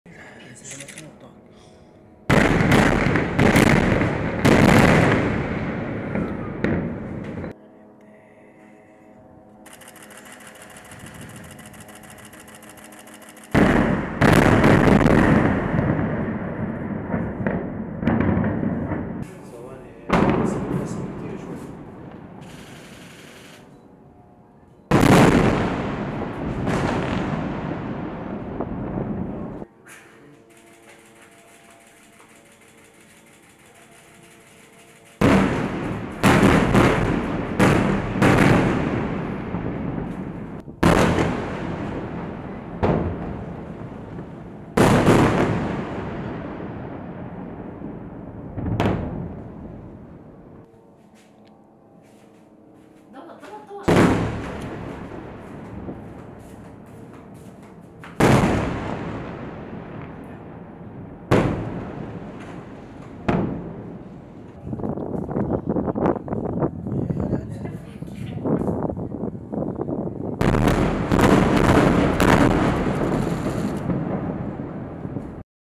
استمرار العدوان الإسرائيلي على غزة لليوم الحادي والثلاثين على التوالي
Israeli air strikes hit Gaza City overnight